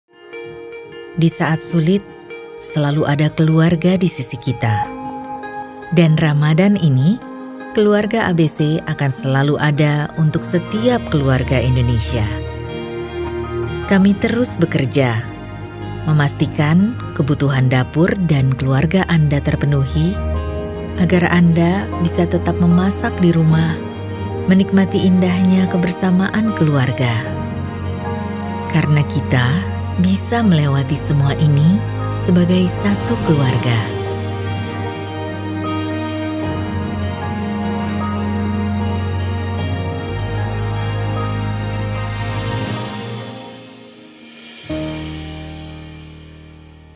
Tetap Bersama ABC di Rumah, Indonesia
Situasi sulit seperti sekarang tidak mengubah komitmen keluarga ABC untuk tetap bekerja memenuhi kebutuhan dapur keluarga Indonesia. Karena kami mengerti akan pentingnya masak di rumah saat ini agar Indonesia bisa tetap menikmati indahnya kebersamaan keluarga, di bulan Ramadan.

Masak bersama di rumah.
Tetap di rumah, Indonesia.